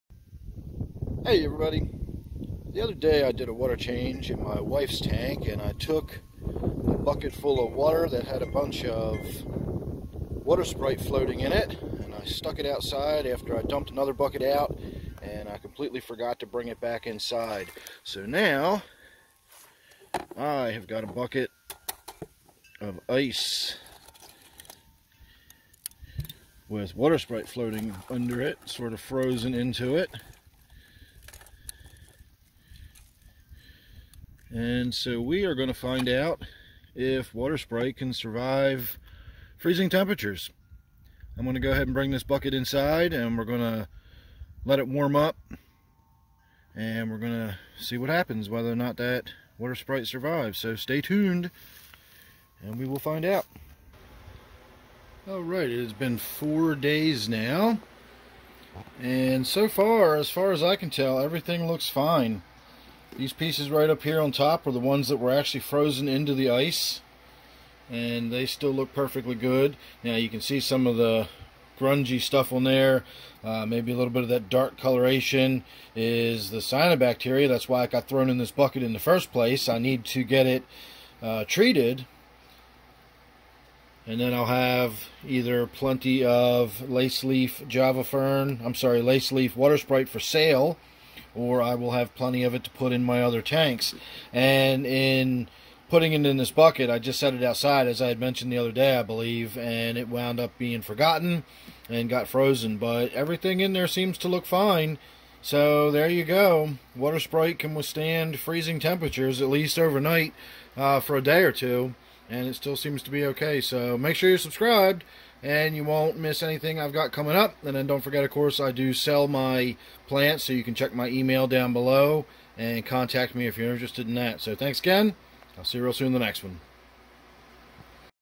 Will Water Sprite Survive Freezing Water?
I left a bucket of lace leaf water sprite outside and the water froze over. Now I'm going to see if it can survive after warming up.